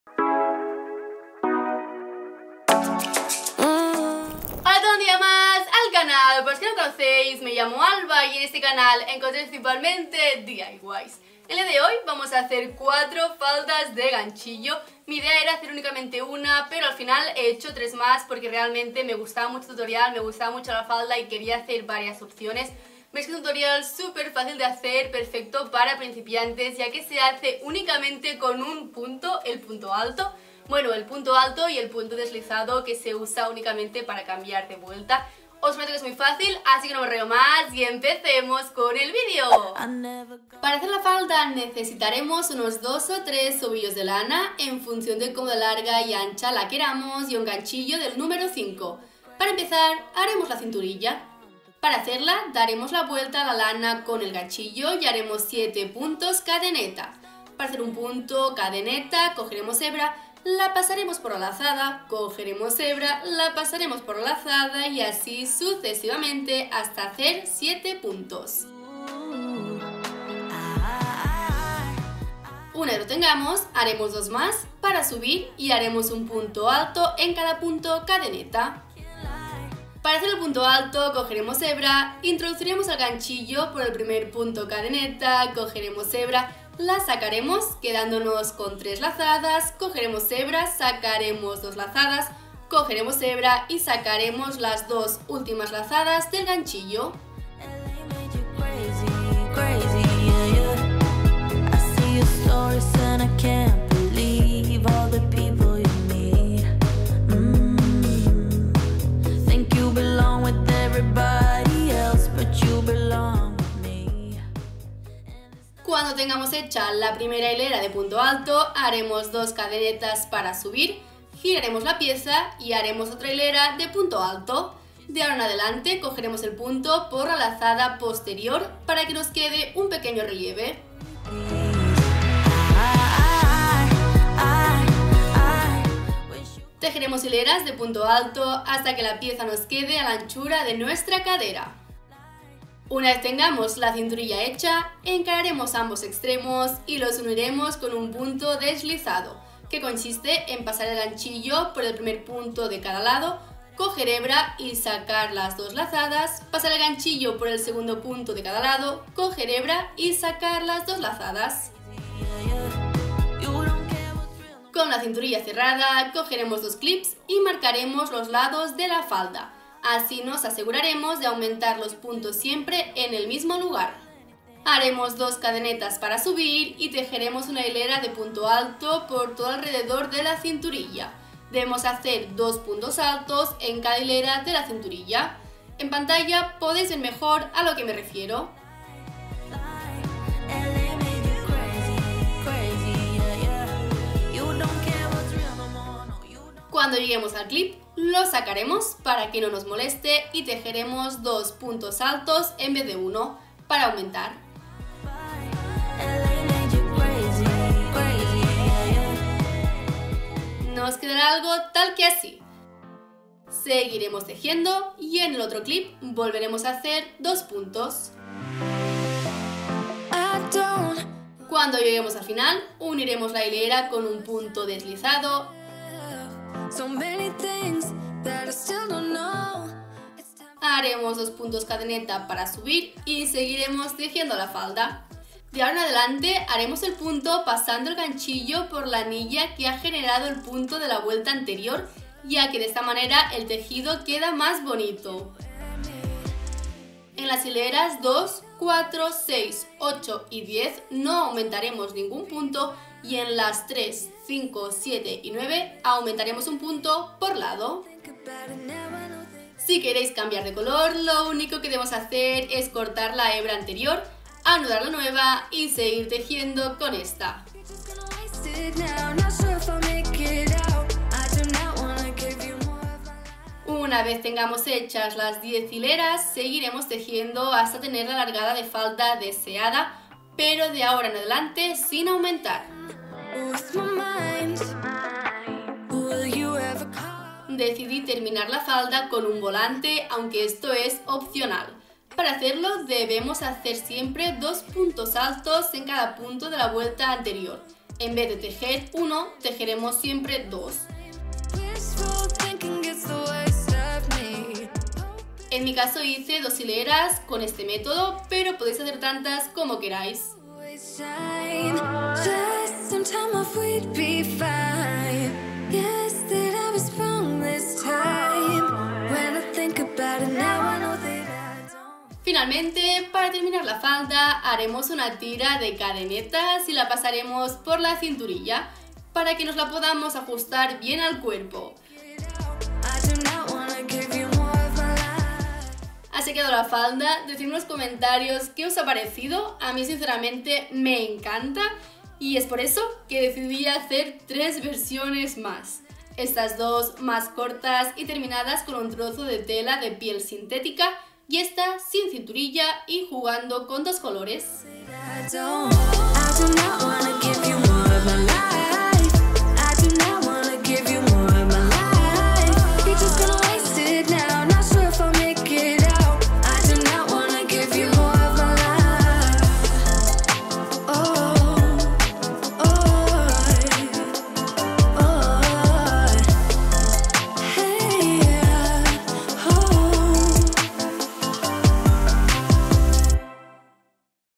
DIY Cómo hacer la FALDA de CROCHET en TENDENCIA💙 *Perfecto para principiantes*
Tutorial de Ganchillo PASO a PASO para aprender a hacer la FALDA de CROCHET en TENDENCIA🧶 *sirve para Todas las Tallas*

92.847 gracias💕 SOIS GENIALES!

0:00 Intro Diy FALDA - Tutorial Paso a Paso Falda de Crochet
0:39 Hemos la Cinturilla
2:44 Tejemos la Falda
5:00 ‎‧₊˚✧ volante Cute ✧˚₊‧ (opcional)
5:22 🎀 lazo para atarnos la falda
5:46 No podía hacer solo una falda🤪
5:55 (try on😁)

📱 Si me queréis conocer un poquito más no olvidéis en seguirme por I N S T A G R A M - @albaselva

📩 Para cualquier cosa, no dudéis en enviarme un correo!